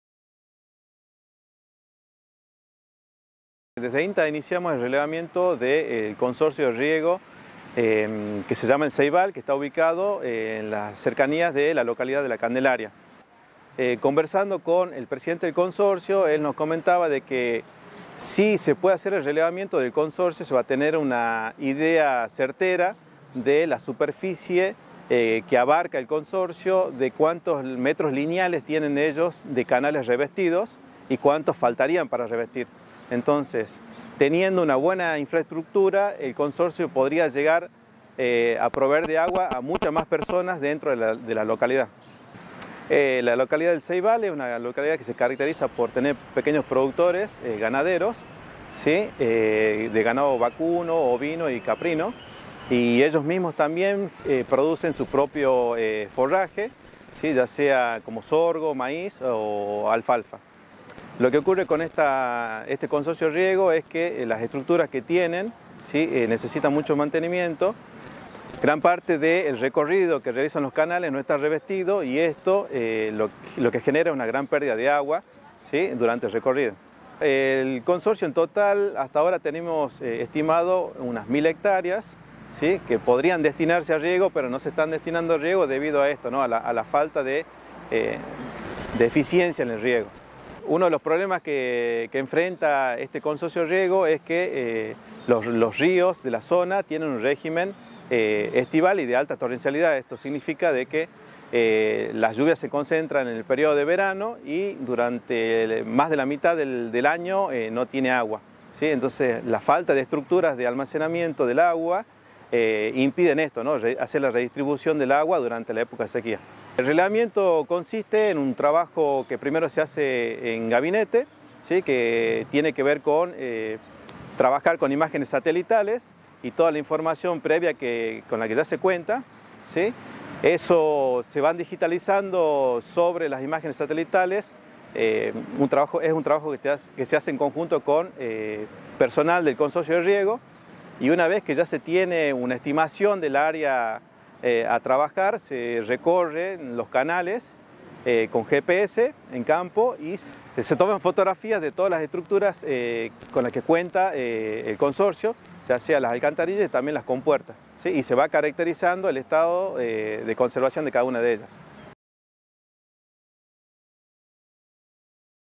Relevamiento del sistema de riego en el Ceibal
Emanuel Visentini  (INTA Metan), nos habla sobre el relevamiento del Consorcio de riego de El Ceibal (Municipio de La Candelaria). Se pretende tener un relevamiento preciso del sistema de riego, tanto su canal de conducción, como el de distribución.
Es indispensable disponer de  información sobre el estado de los canales, para así, ser mas eficientes en el riego y el almacenamiento de agua.